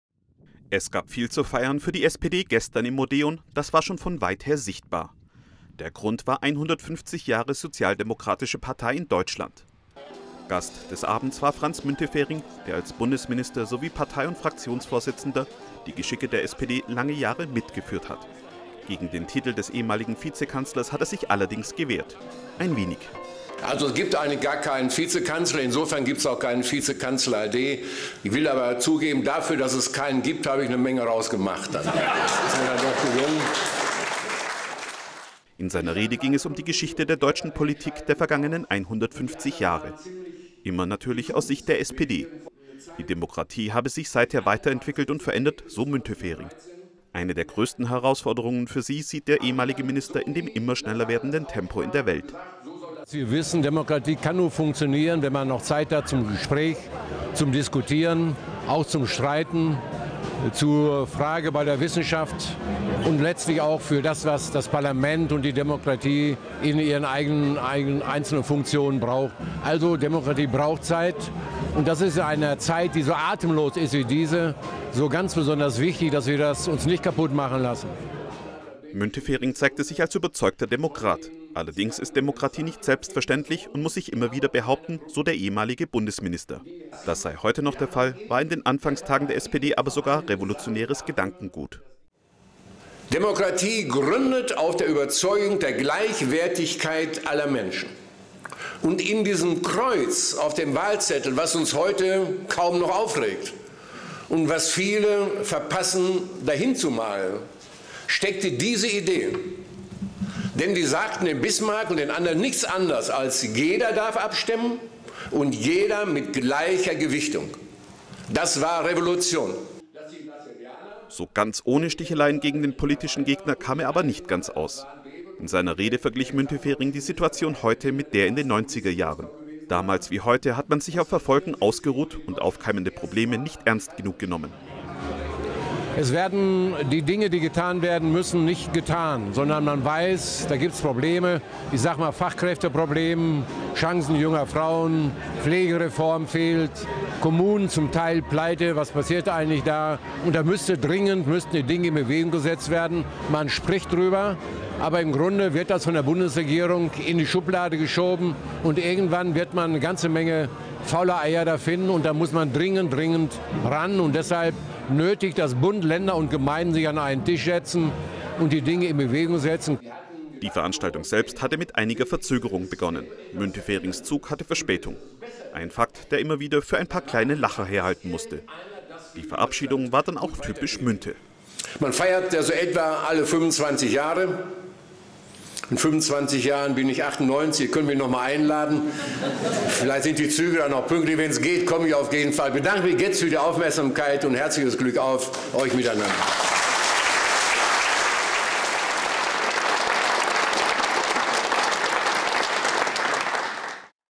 150 Jahre deutsche Sozialdemokratie: SPD-Ostallgäu feiert das Jubiläum mit Franz Müntefering
Franz Müntefering war in Marktoberdorf zu Besuch. Es war kein Wahlkampfauftritt, zumindest keiner im klassischen Stil. Statt dessen sprach der scheidende Bundestagsabgeordnete sowie langjährige SPD-Führungsfigur über die Geschichte der Sozialdemokratie. Immerhin feiern die Genossen dieses Jahr den 150. Geburtstag ihrer Partei. Bei der Rede ging es vor allem um die Politik und Geschichte Deutschlands aus sozialdemokratischer Sicht. Vorgetragen wurde sie nach der typischen Art von Müntefering: trocken, knarzig und mit einem Schuss Selbstironie.  | von Steffen Konrad / Julian Küpper